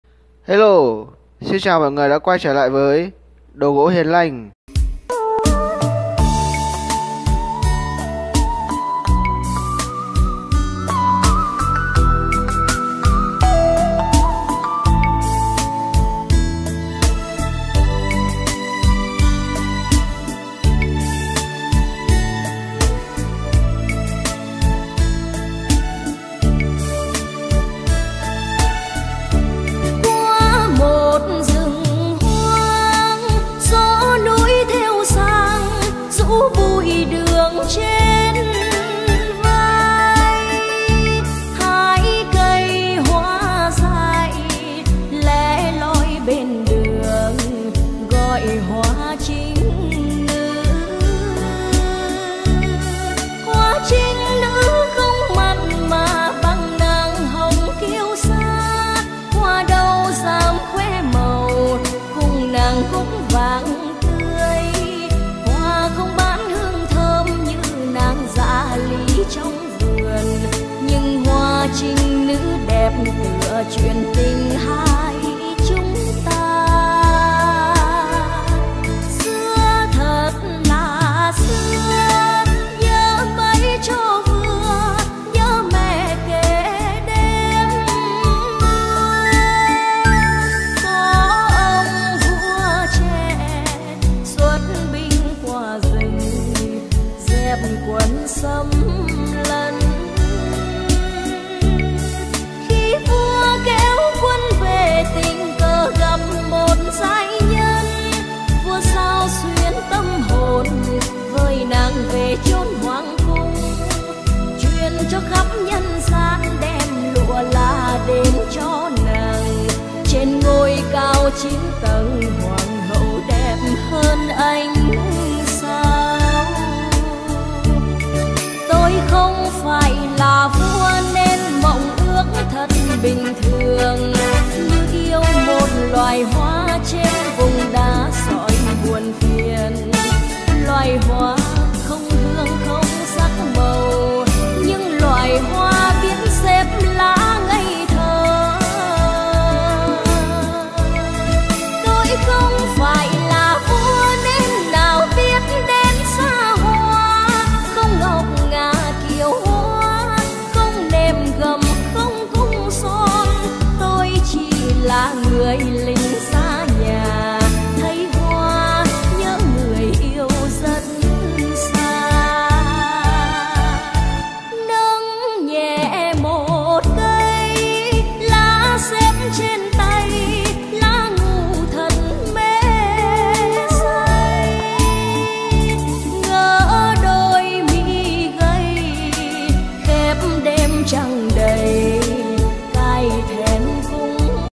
Bộ bàn ghế gỗ mun hoa Thật| bộ bàn ghế ăn gỗ mun hoa chọn vân. đồ gỗ Hiền Lanh. 0986883349
bộ bàn ghế ăn gỗ mun hoa chọn vân, mẫu hoa lan tây - hàng đặt a Cường( Vũng Tàu)
trên thị trường hiện nay người mua hàng cảnh giác cao với gỗ mun hoa giả - giá rẻ - hàng kém chất lượng